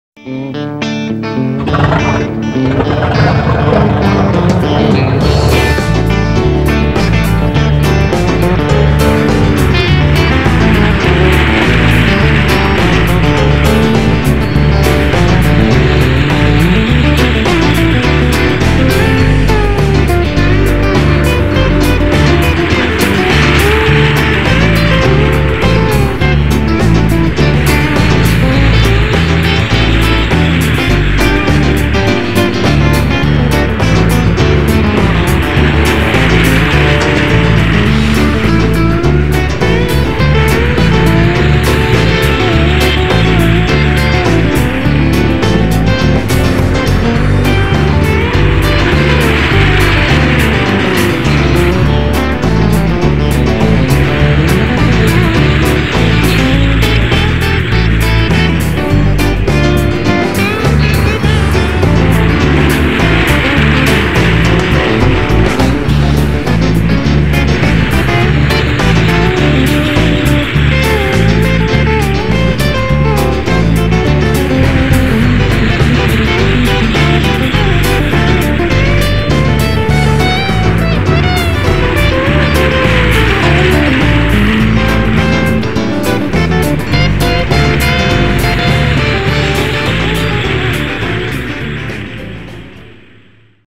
JOHN & SHIRLEY FLY 1961 -- by John E. Fulton
At the age of 20 years John & Shirley fly around the Ontario/Pomona, Calif. area in 1961. Photos & video by John E. Fulton